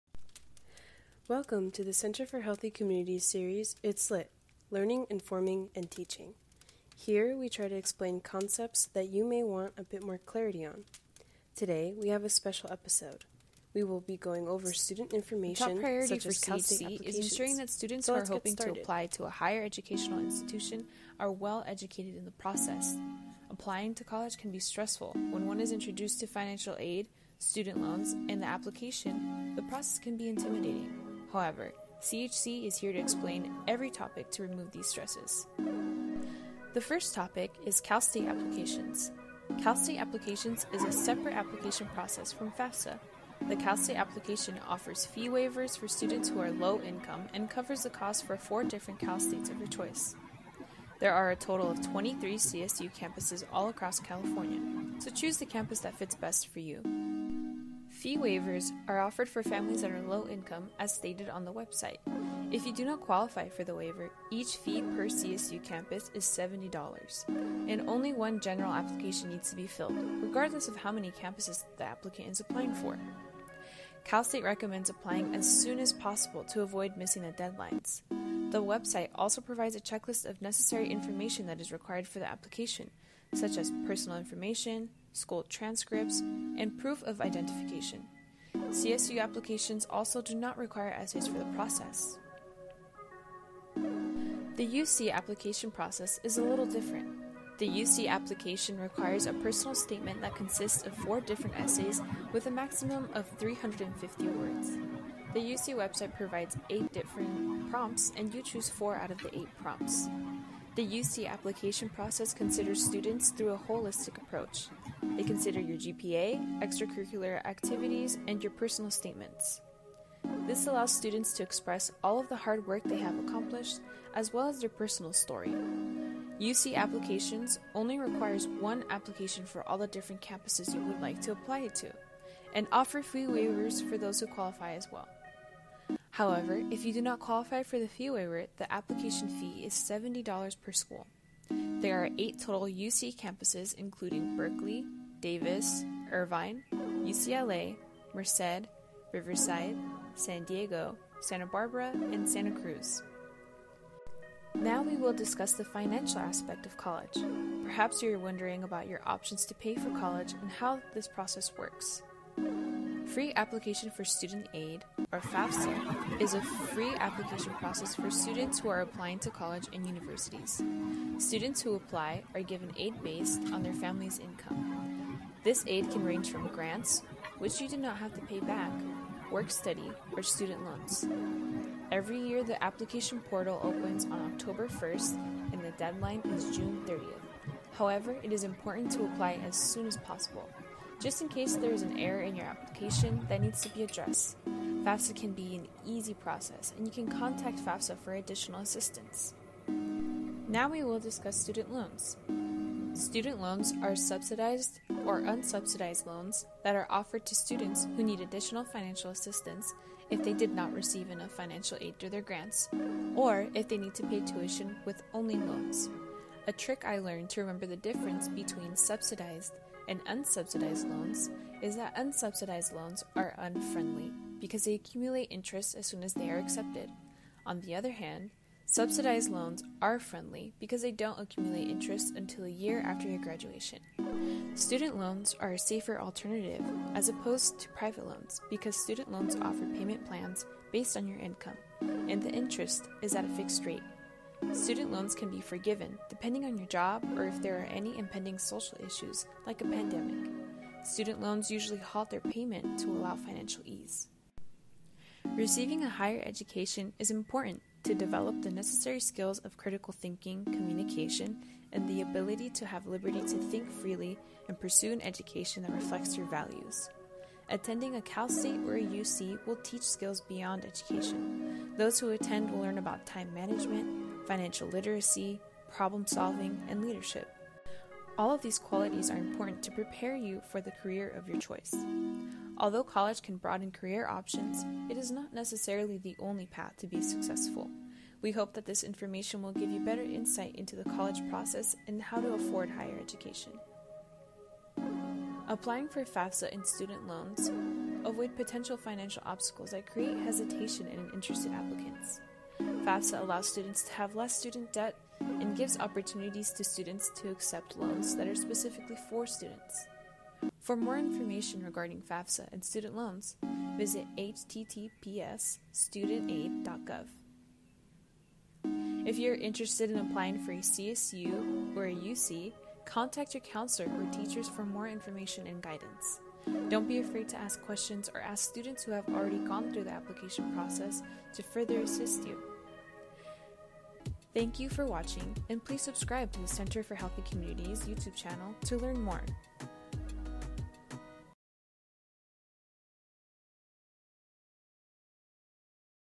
It's L.I.T. - College Prep: California Applications
Join UCR School of Medicine's Center for Healthy Communities on their series, It's L.I.T, Learning, Informing, & Teaching. In this episode, we go over California Universities application and Fasfa .

Featured voice: Briana Dominguez

For Federal Student Aid Application visit: studentaid.gov
For CSU information visit: calstate.edu/apply
For UC information visit: admission.universityofcalifornia.edu